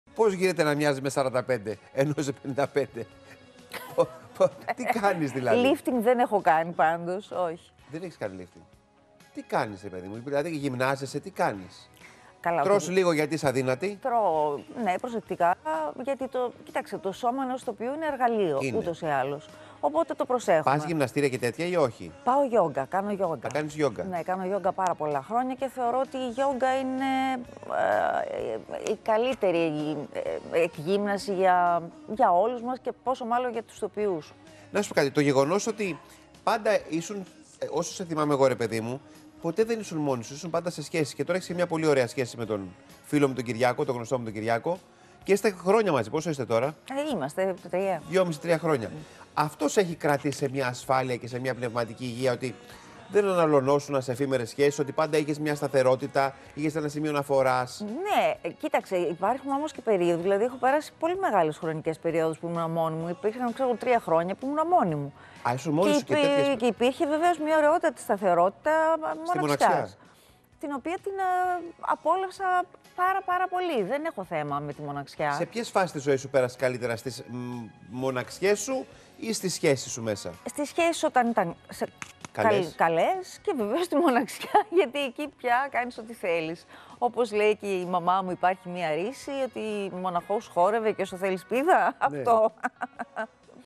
Γιώργος Λιάγκας σε Αλεξάνδρα Παλαιολόγου: "Πώς γίνεται να μοιάζεις με 45 ενώ είσαι 55;"
Η αντίδραση της ηθοποιού...